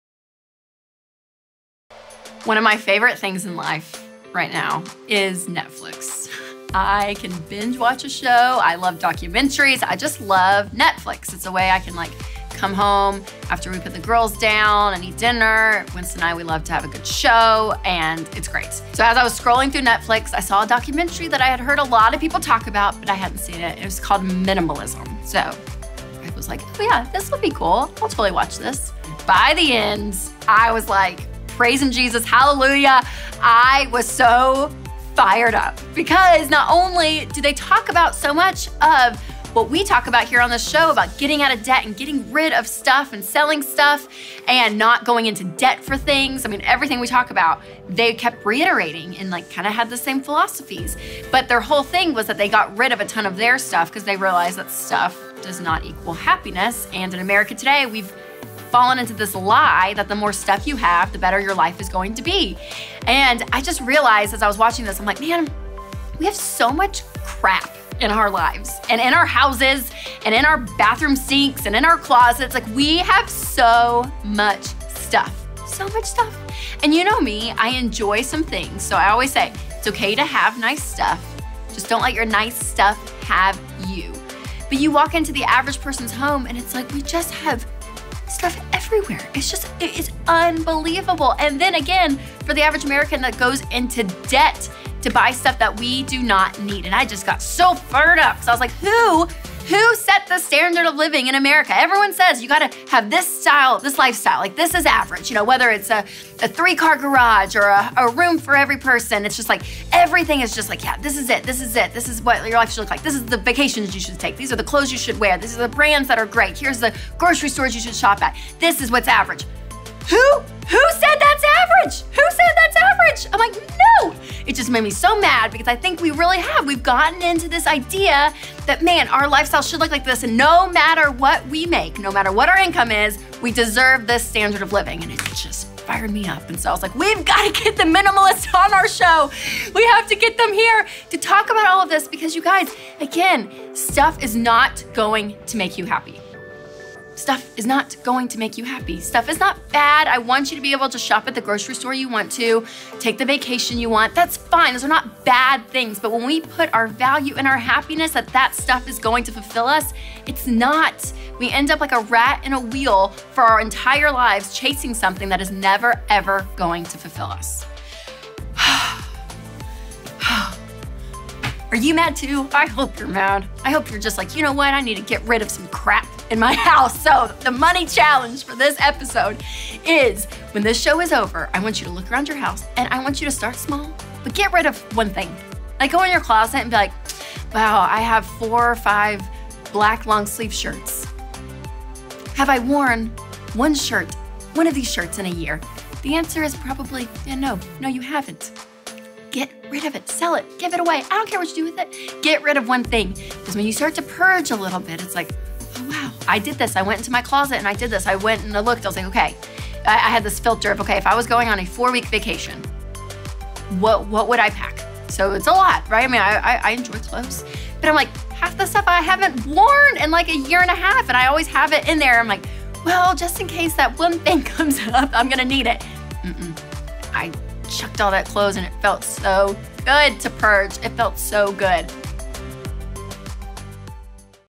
Why Stuff Won’t Make You Happy
Who set the standard of living in America today anyway? Who decided that you must have the latest and greatest of everything to be happy? I’m getting mad again just thinking about it!

Get even more practical tips or encouragement for getting out of debt, budgeting, and creating a life you love, check out my book Love Your Life, Not Theirs